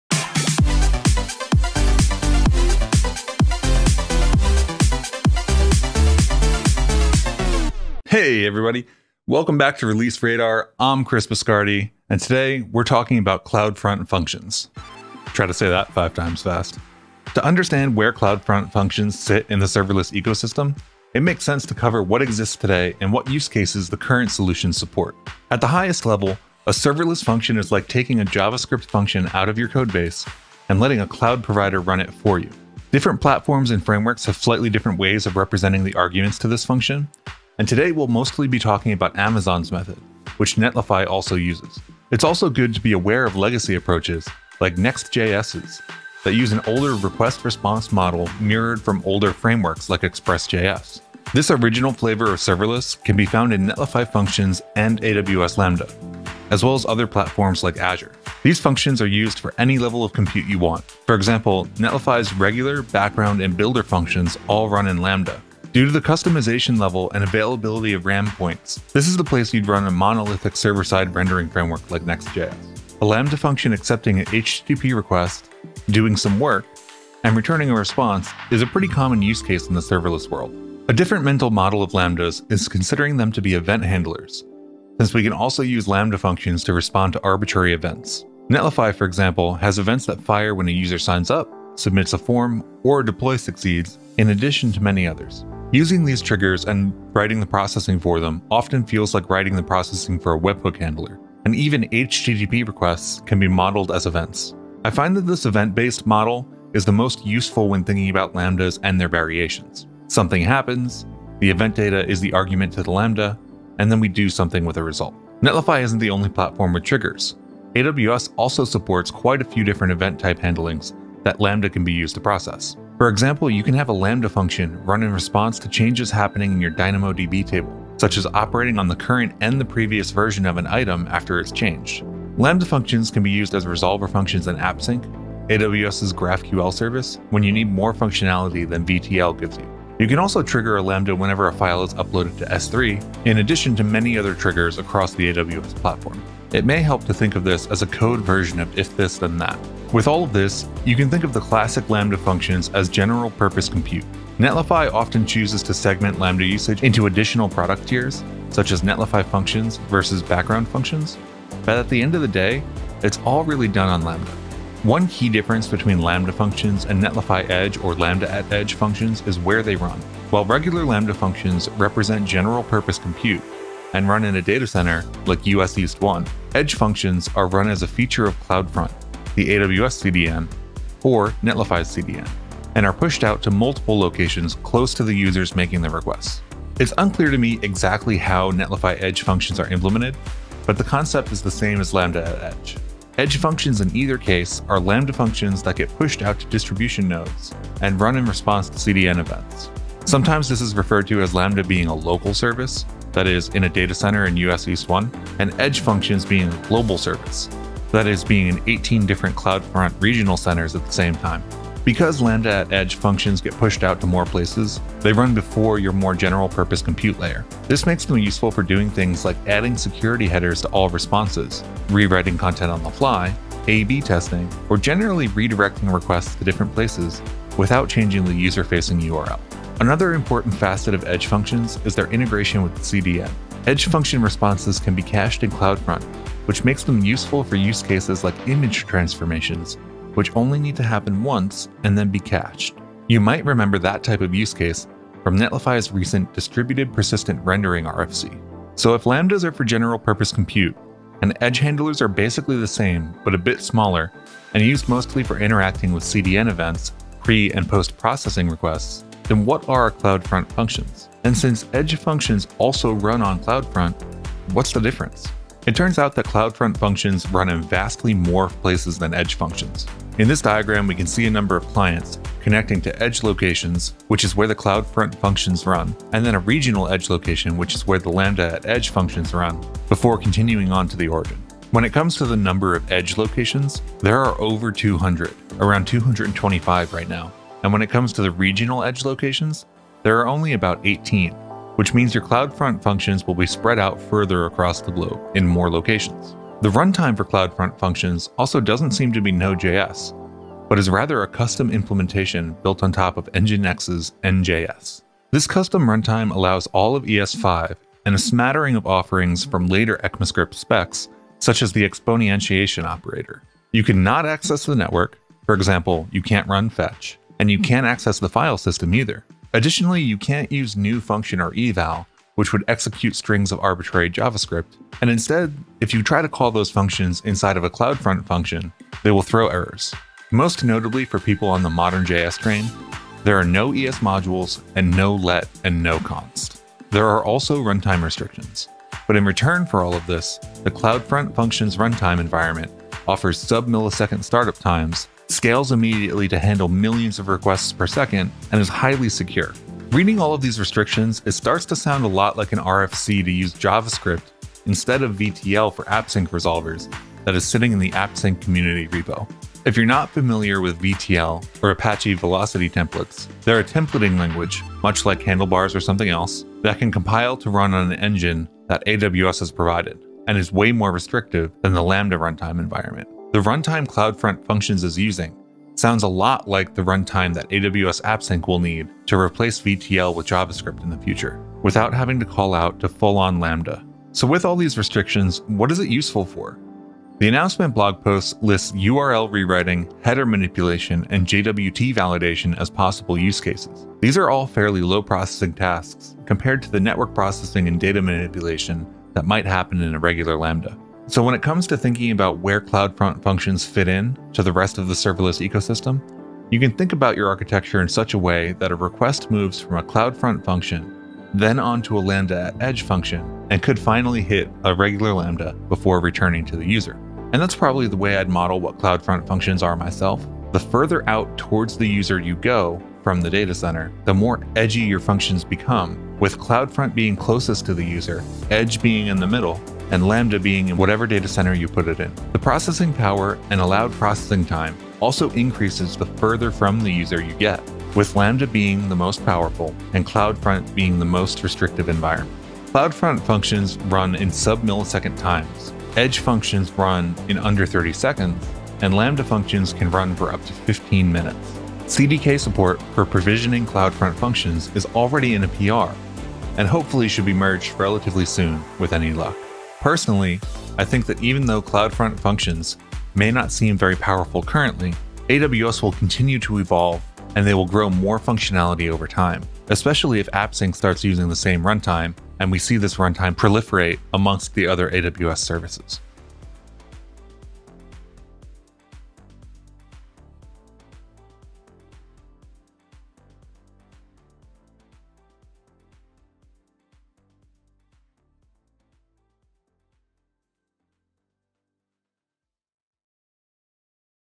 AWS CloudFront Functions - Release Radar
Introducing CloudFront Functions – Run Your Code at the Edge with Low Latency at Any Scale

## Timestamps

00:00:00   Intro
00:00:25   What is Serverless anyway?
00:01:20   The Event Model
00:02:48   Lambda vs Edge
00:04:18   What are CloudFront Functions
00:07:31   CDK and future of CF Functions